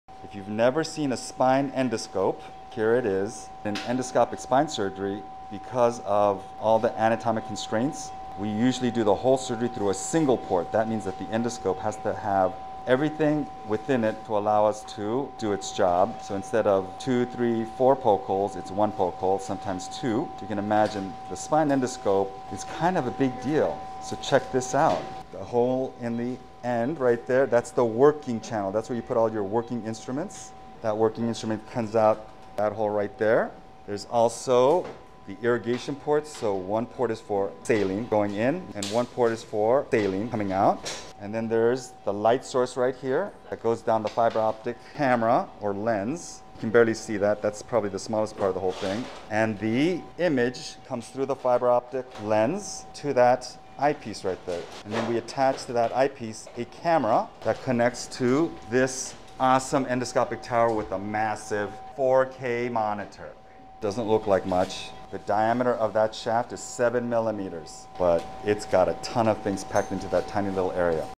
Meet the Endoscope 👀
Ever wondered how endoscopic spine surgery works? It’s impossible without the endoscope… a state-of-the-art tool that lets us perform precise, targeted procedures with small incisions, meaning less pain and no damage to the important muscles leading to a faster recovery for patients!!!